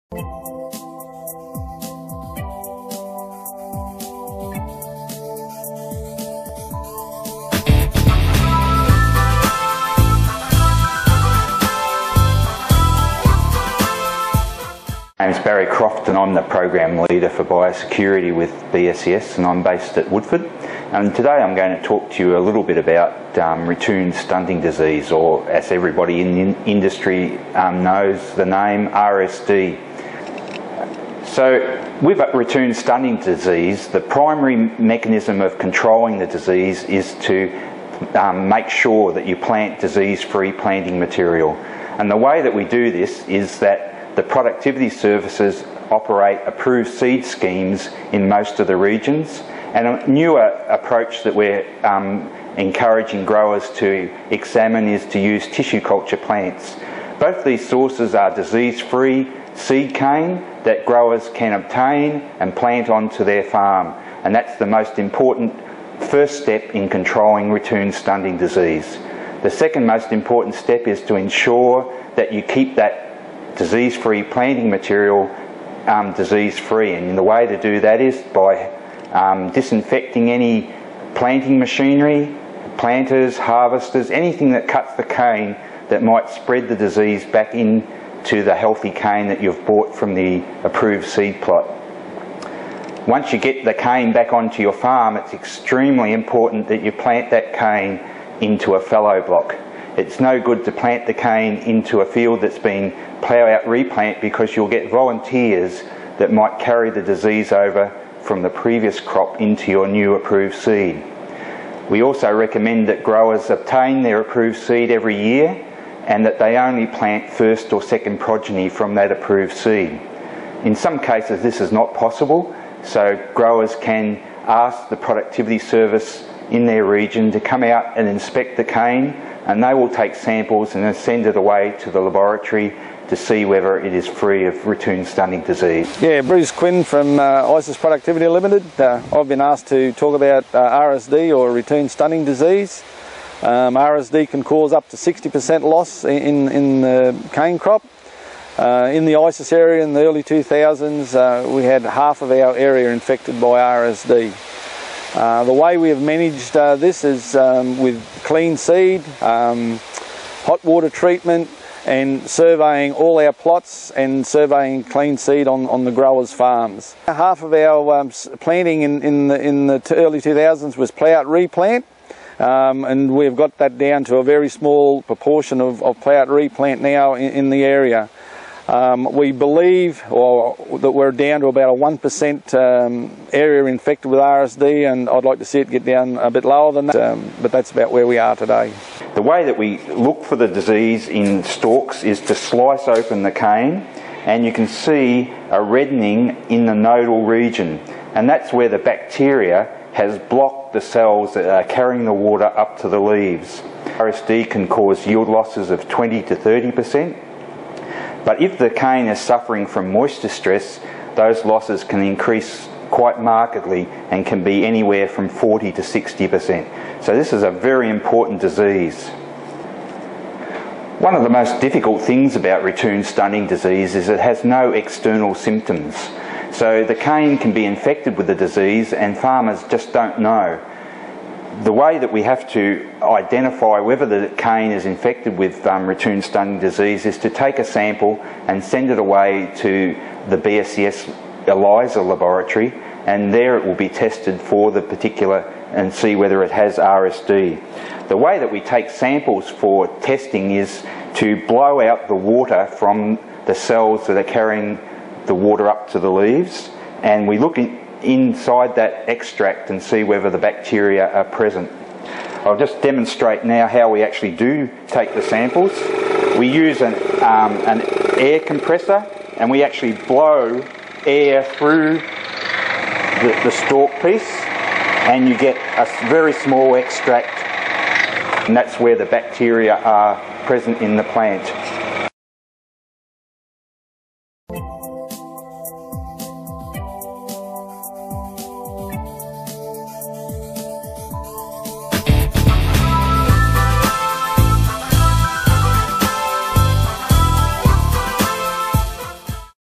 CaneClips RSD Jul2013
Controlling Ratoon Stunting Disease and why it's so important.